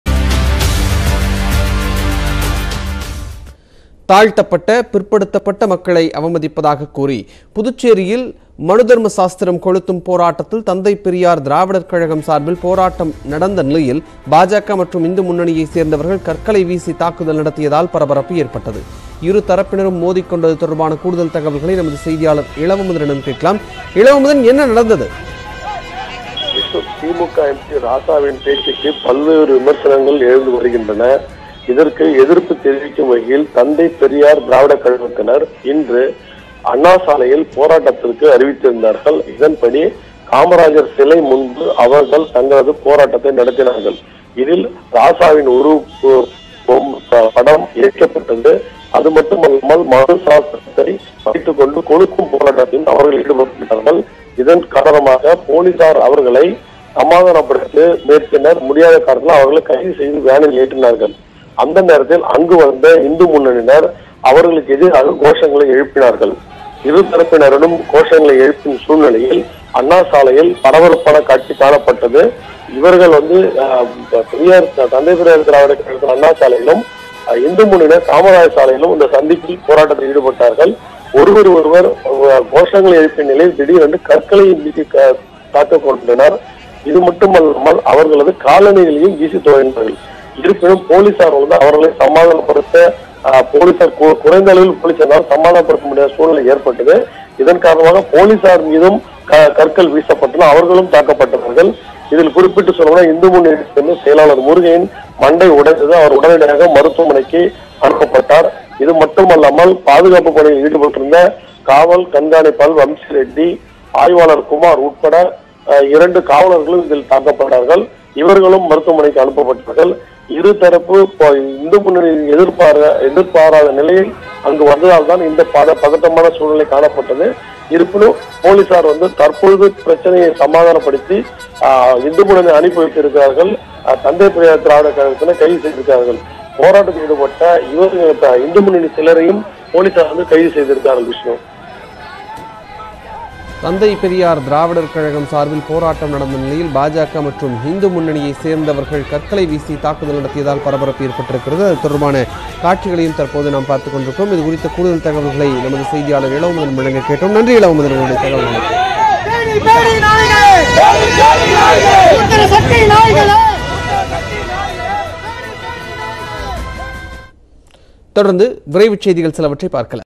JUST NOW | தந்தை பெரியார் திராவிடர் கழகம், பாஜகவினர் இடையே மோதல் | Puducherry
JUST NOW | புதுச்சேரியில் தந்தை பெரியார் திராவிடர் கழகம், பாஜகவினர் இடையே மோதல் | செய்திகளை துல்லியமாகவும் துரிதமாகவும் வழங்கும் தமிழகத்தின் முதன்மை செய்தி தொலைக்காட்சி உங்கள் நியூஸ் 18 தமிழ்நாடு JustNow News18TamilNadu puducherry bjp thanthai periyar dravidar kazhagam#TamilNews n18oc_tamil-nadu

Tue Sep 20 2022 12:40:59